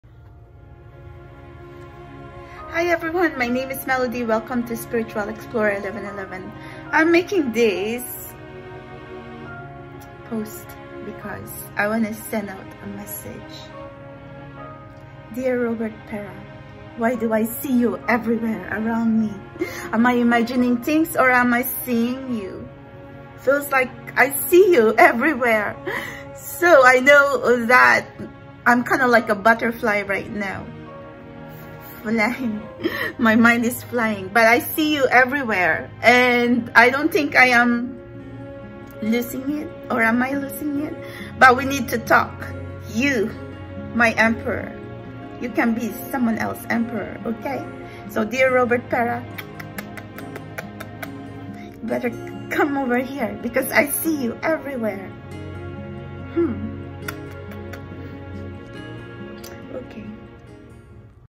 DEAR ROBERT PERA , MY SOULMATE , MY EMPEROR🤴, WHY DO I SEE YOU EVERYWHERE 🦋💍♥️😘💐?
Robert Pera is my first spiritual download from Universe.  He became a big part of my Spiritual Journey. In my darkest moments, he made me see the light .

I truly pray he will come my way 🦋.

Melody